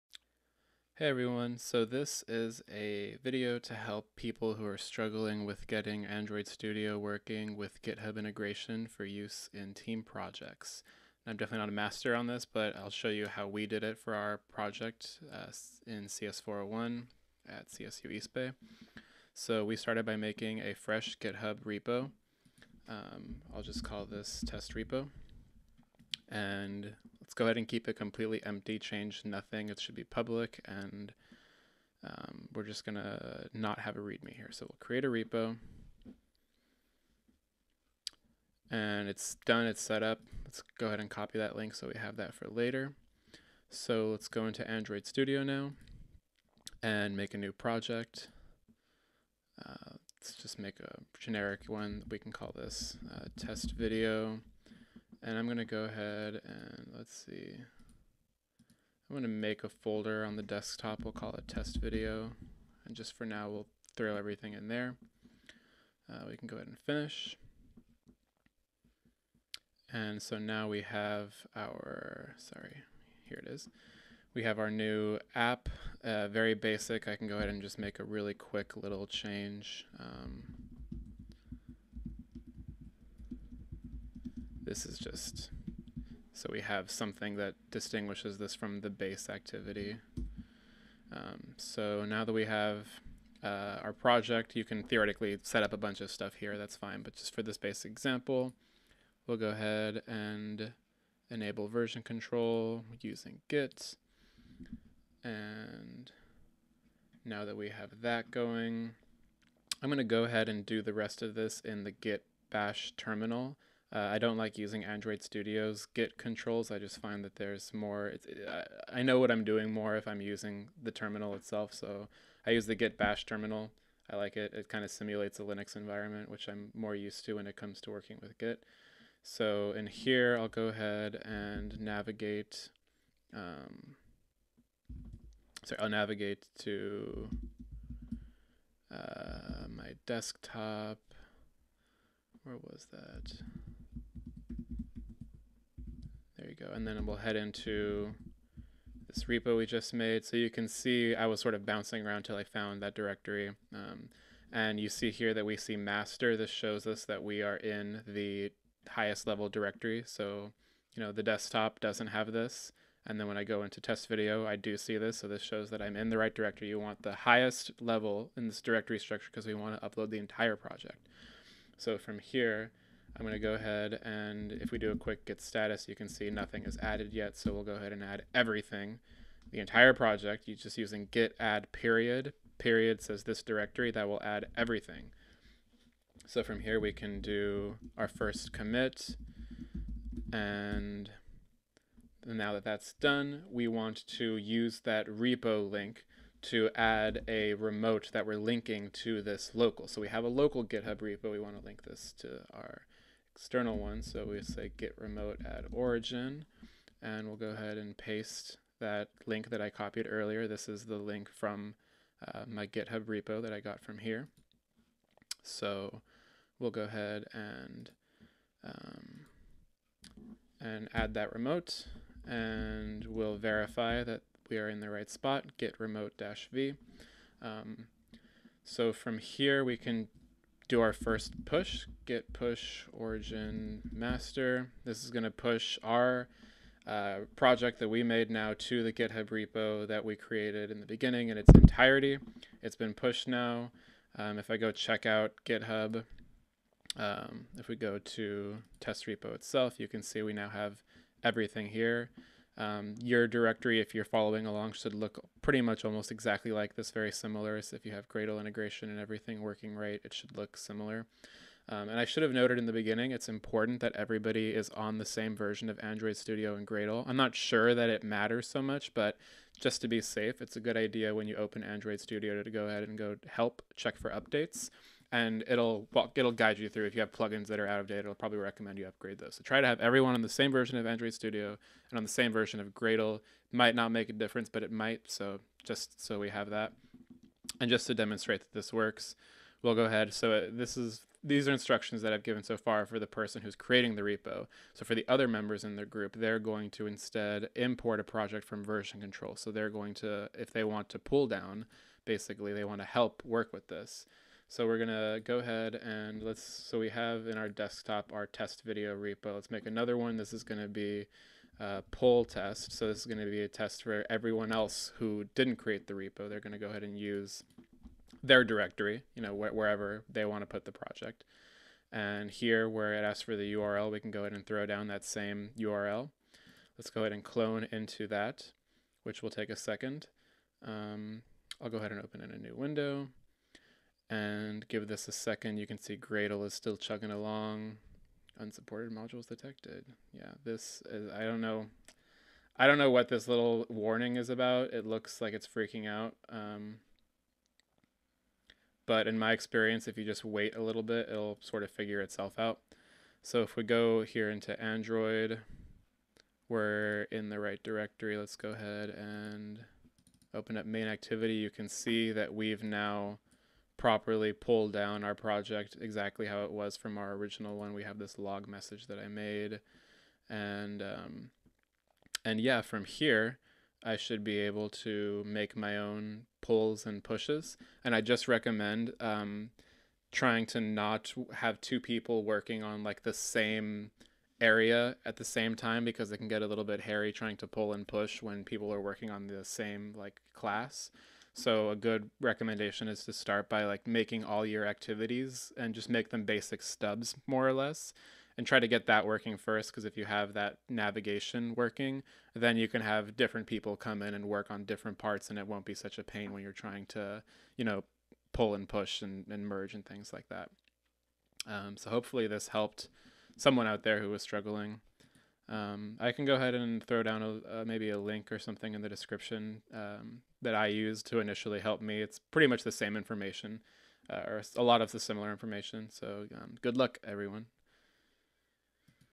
AndroidStudio w/Git+GitHub Integration for Team Projects
Quick tutorial on setting up a local project, adding integration, and using it to build a remote git repo that others can push/pull from as well. Much of theinfo was taken from here with some modifications for adapting it to work AndroidStudio: